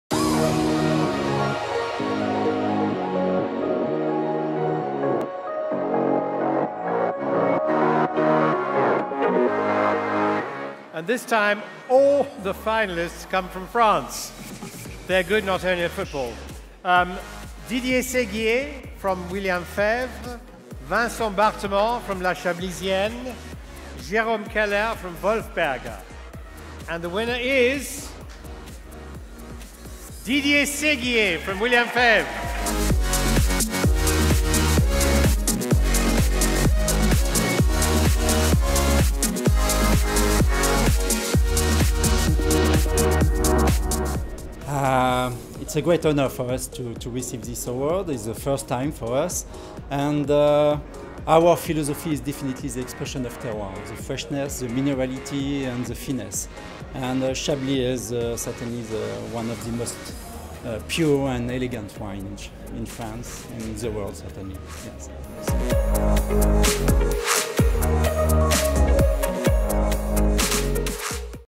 IWC White Winemaker of the Year 2018 - Didier Séguier from William Fèvre
The IWC White Winemaker of the Year 2018 is Didier Séguier.

Every wine that William Fèvre entered into the IWC 2018 was awarded a medal, in total they received: 7 Golds, 10 Silvers and 8 Bronzes.